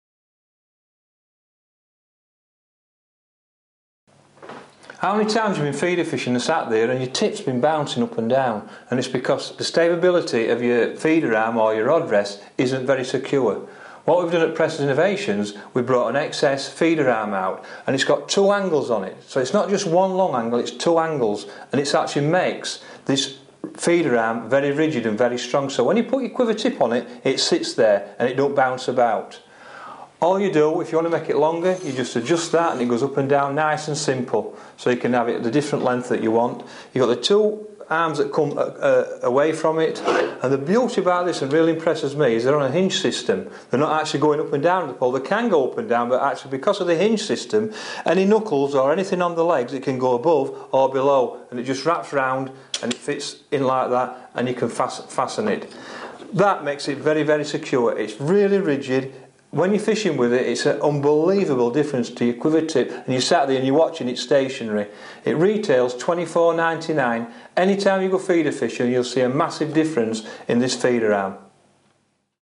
Tackleuk Preston Innovations XS Feeder Arm
Designed around a new clamp style knuckle, the XS Feeder Arm can be fitted below the frame of your seat box, or below other accessories. The two fixing points make this arm the most stable we have produced, whilst still remaining infinitely adjustable. The inner tube is D sectioned in profile to completely eliminate any rotation on the arm. Complete with a full range of inserts to enable it to be used on most popular seat boxes.